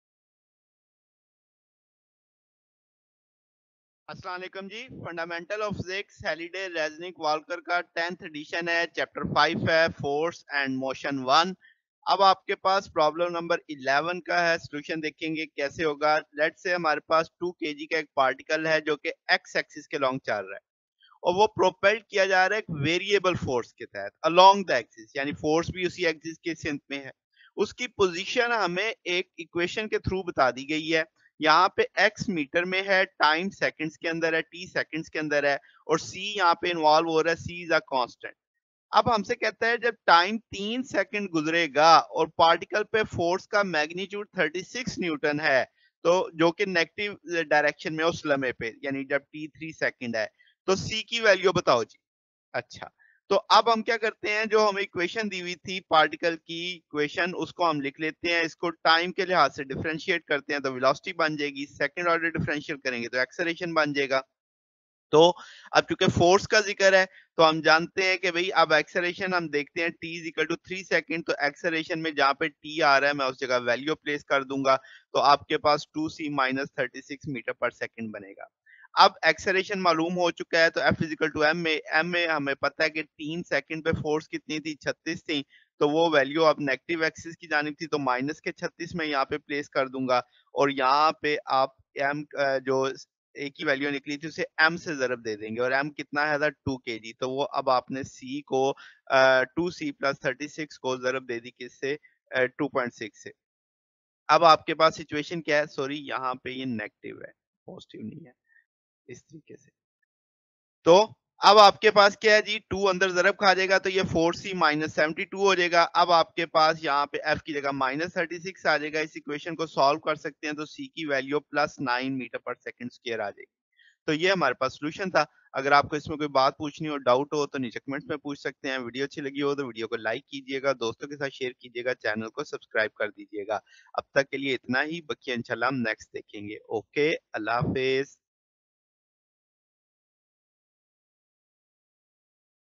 Halliday resnick chapter 5 problem 11 solution | Fundamentals of physics 10e solutions
A 2.0 kg particle moves along an x axis, being propelled by a variable force directed along that axis, Its position is given by x=3.0 m+(4.0 m/s)t+ct2-(2.0 m/s3 )t3 , with x in meters and t in seconds, The factor c is a constant, At t=3.0 s, the force on the particle has a magnitude of 36 N, and is in the negative direction of the axis,What is c?,